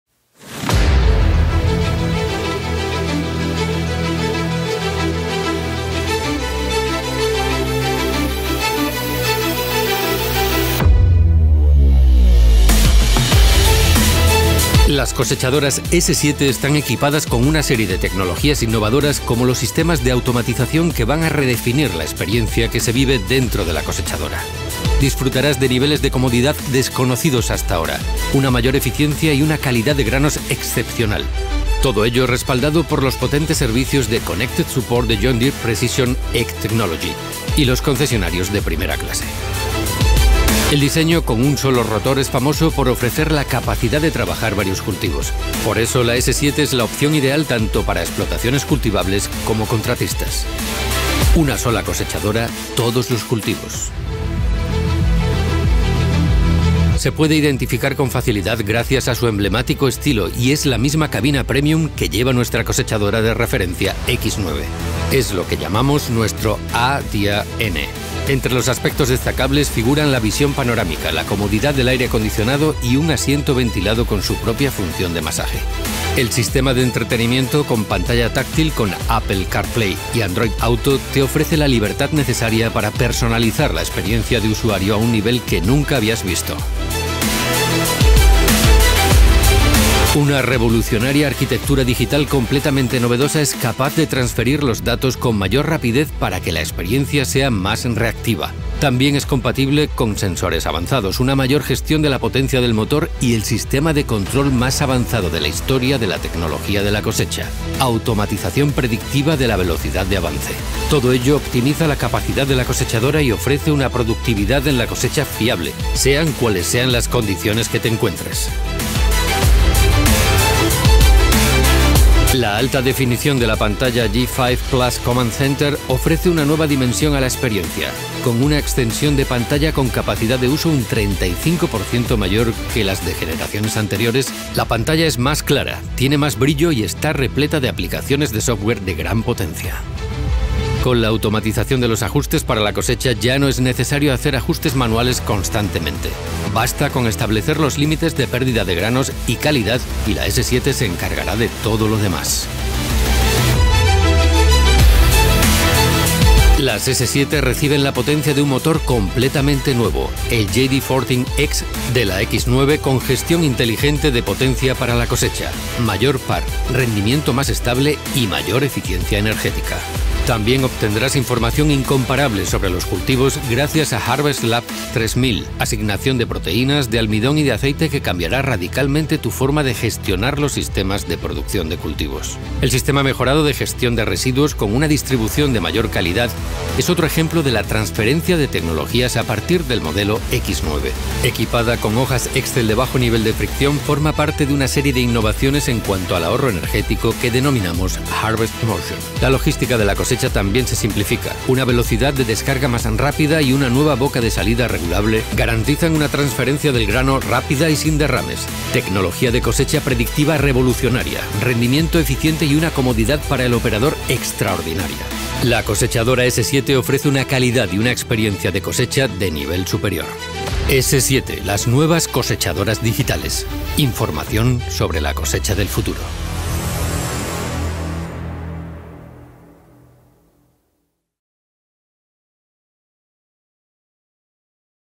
🌾 La nueva S7 marca el comienzo de una nueva era en la evolución de las cosechadoras.
🟢John Deere ha presentado su nuevo modelo de cosechadora S7.

🌾La nueva S7 marca el comienzo de una nueva era en la evolución de las cosechadoras. Descubre la innovadora tecnología de cosecha predictiva.

Su diseño, su eficiente motor JD9X (S7 700) y JD14X ( S7 800, S7 850 &S7 900) ofrecen una eficiencia de combustible sin precedentes, cumplen con la fase V y producen más potencia cuando es necesario con gestión inteligente de potencia Harvest IPM. Su tecnología de vanguardia garantizan una experiencia de cosecha espectacular y resultados excepcionales.

👉Disfruta de la máxima productividad y eficiencia con las cosechadoras S7 de Agrícola Castellana.